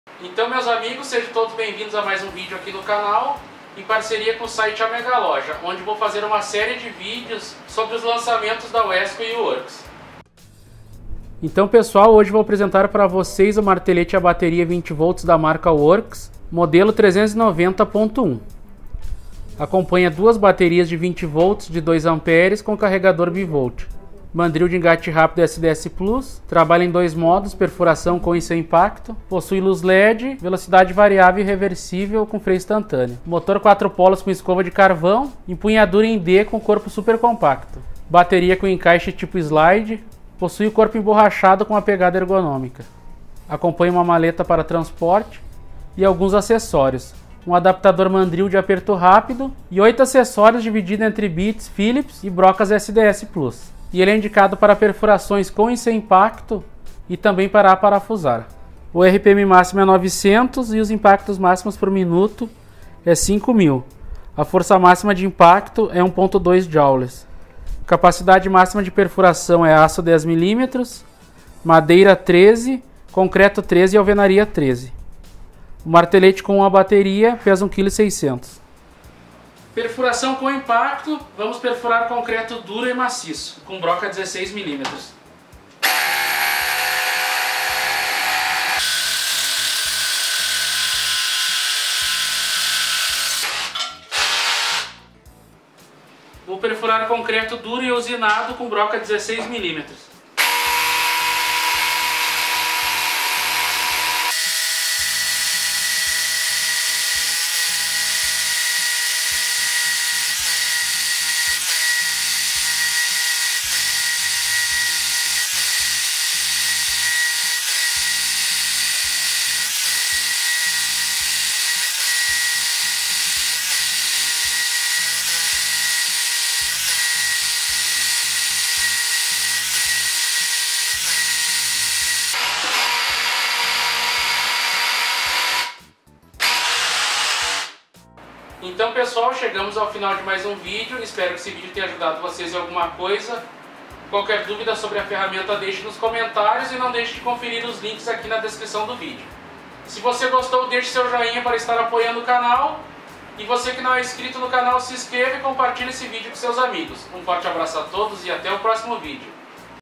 Martelete SDS Plus 20V WX390.1 Worx - A Mega Loja
Martelete SDS Plus à bateria 20V lítio WX390.1 da Worx

Poderoso no concreto, o H3 WX390.1 é surpreendentemente leve e compacto, além de ser a ferramenta mais ergonômica da categoria. Ele é 3 em 1: Martelete SDS-Plus, Furadeira e Parafusadeira, sendo ideal para aplicações críticas onde os marteletes convencionais poderiam ser desajeitados e pesados demais para a atividade, como trabalhos acima da cabeça, por exemplo.

Especificações técnicas:

- Bateria: 20 V / 2.0 Ah Li-íon;
- Velocidade sem carga: 0 - 900 /min;
- Impactos: 0 - 5000 bpm;
- Energia de impacto: 1.2 J;
- Tempo para carregar bateria: 1 hr;
- Peso da máquina: 1.6 kg.

Capacidade máxima de perfuração:

- Madeira 13mm;
- Concreto 13mm;
- Aço 10mm;
- Concreto/Alvenaria 13mm.

Quer adquirir o Martelete? e confira!

Gostou do vídeo? Não se esqueça de clicar em "Inscrever-se" para receber todos os vídeos novos!